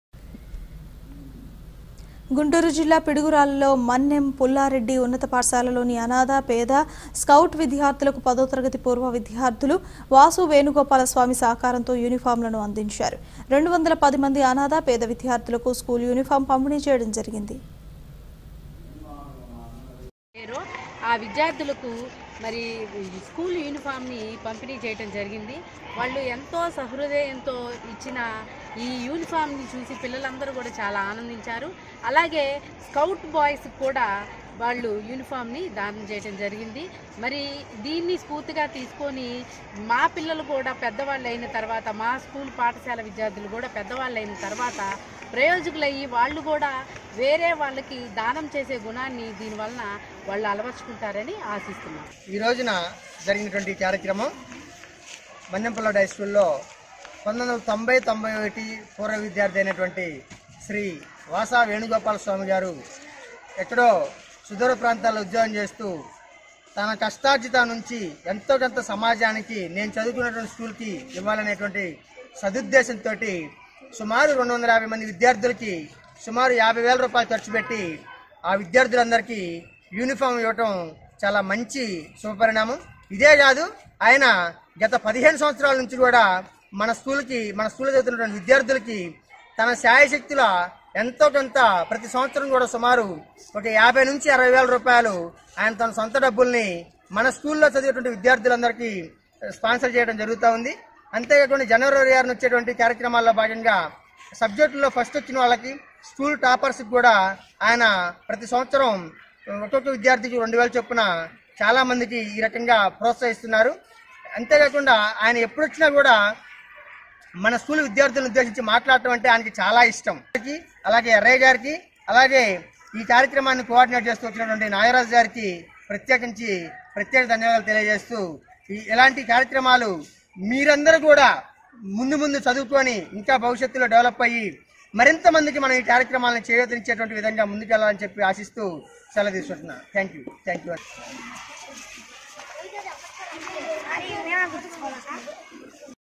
పిడుగురాళ్లలో మన్నెం పుల్లారెడ్డి ఉన్నత పాఠశాలలో పేద విద్యార్థులకు యూనిఫామ్‌ల‌ను అందించారు
గుంటూరు జిల్లా  పిడుగురాళ్లలో   మన్నెం  పుల్లారెడ్డి   ఉన్నత పాఠశాలలోని అనాథ,  పేద ,  స్కౌట్ విద్యార్థులకు  ప‌దో త‌ర‌గ‌తి పూర్వ విద్యార్థులు  వాసు వేణుగోపాల స్వామి స‌హ‌కారంతో యూనిఫామ్‌ల‌ను అందించారు.  210 మంది అనాథ‌, పేద విద్యార్థుల‌కు స్కూల్ యూనిఫామ్ పంపిణీ చేయ‌డం జ‌రిగింది.